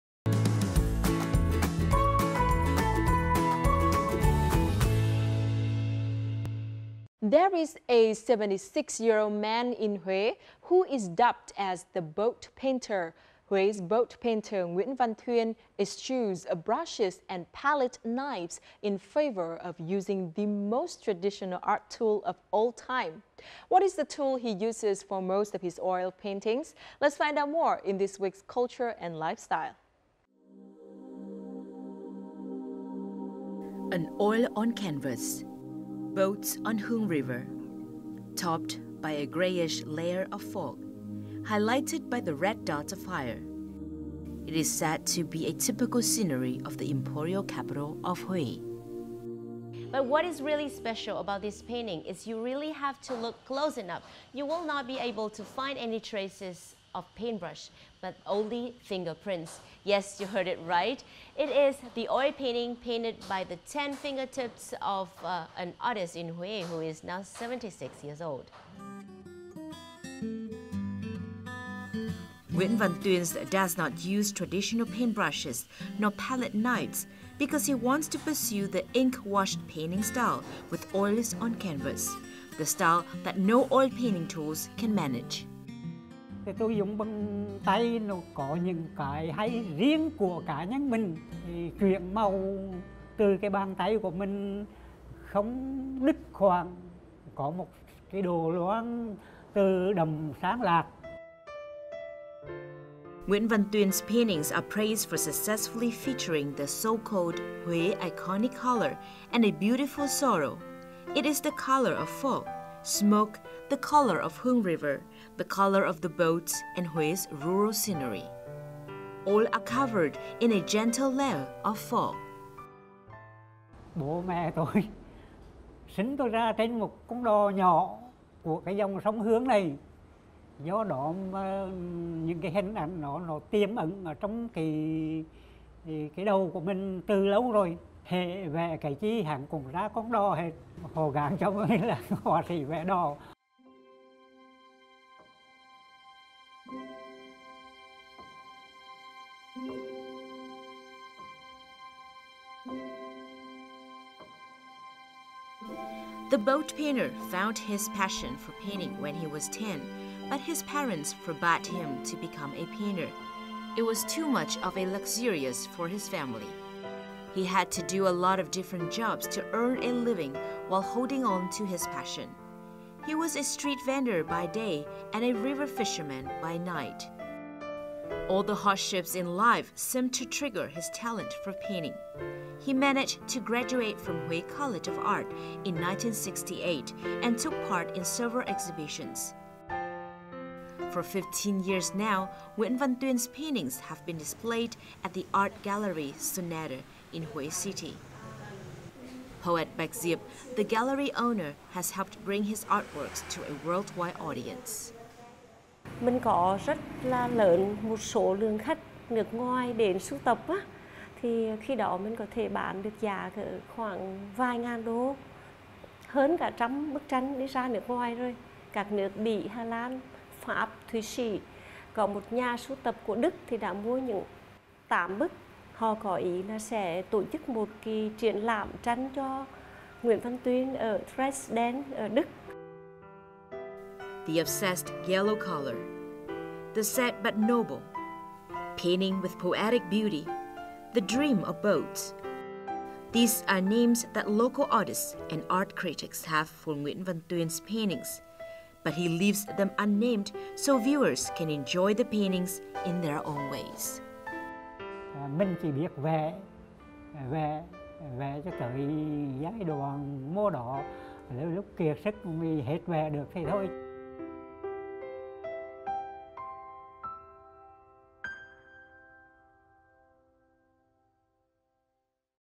Finger painting with oils | VTV World
There is a 75 year-old man in Hue – who is dubbed as “The Boat Painter”. Hue’s “boat painter” Nguyễn Văn Thuyên eschews brushes and palette knives in favor of using the most traditional art tool of all time.
What is the tool he uses for most of his oil paintings?